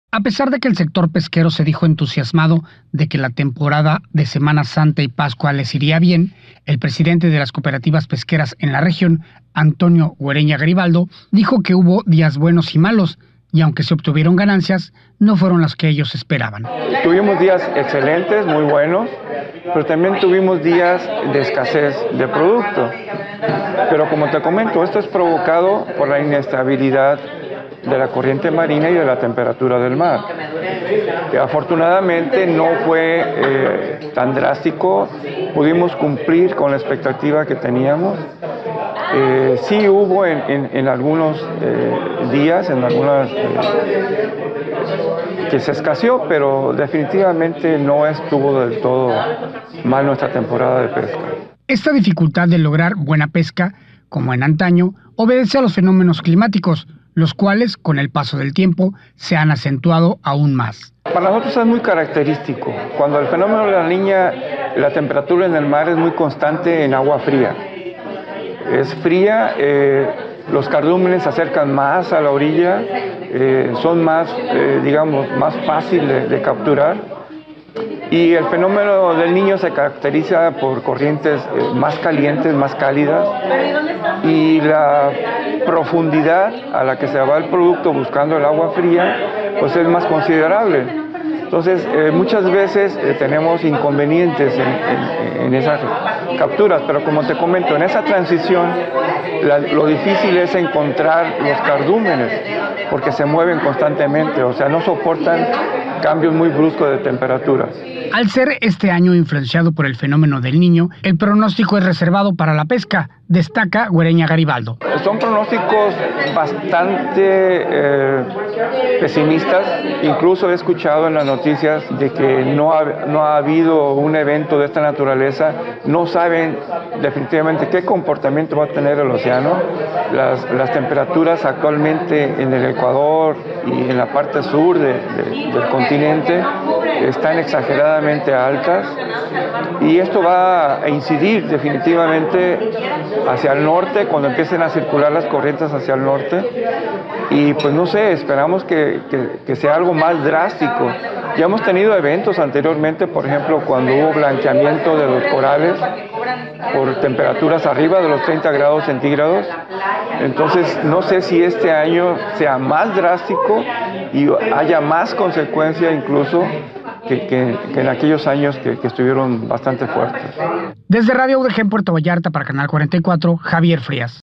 El sector pesquero ha tenido una difícil temporada, debido al fenómeno Del Niño
El sector pesquero ha tenido una difícil temporada, y el resto del año se avizora de pronóstico reservado debido a la presencia del fenómeno Del Niño.